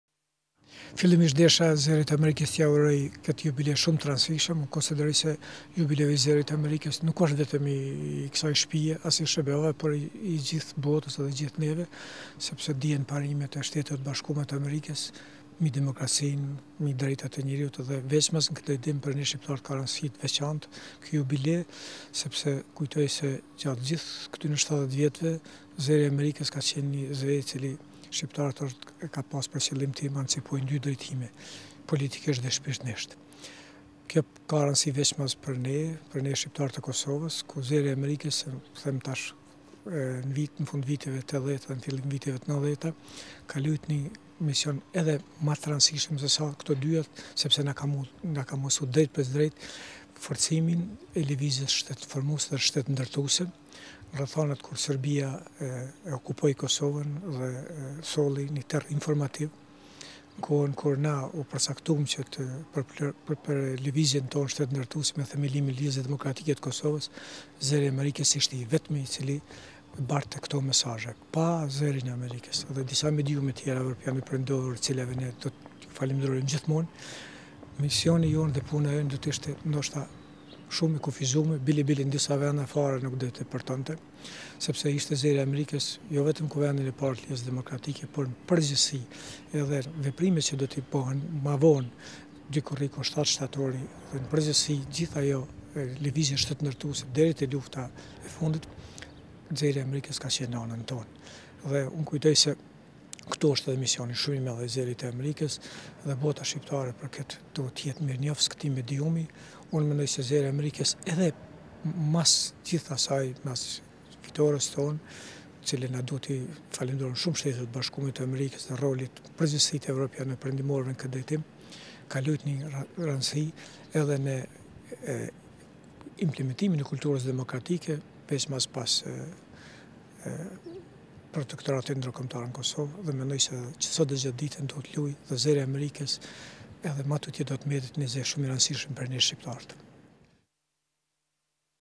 Shkrimtari Jusuf Buxhovi uron Zërin e Amerikës
Shkrimtari Jusuf Buxhovi në urimin e tij për 70 vjetorin e Zërit të Amerikës tha se Zëri i Amerikës ka luajtur një rol të rëndësishëm në implementimin e kulturës demokratike në Kosovë pas shpalljes së pavarësisë së saj.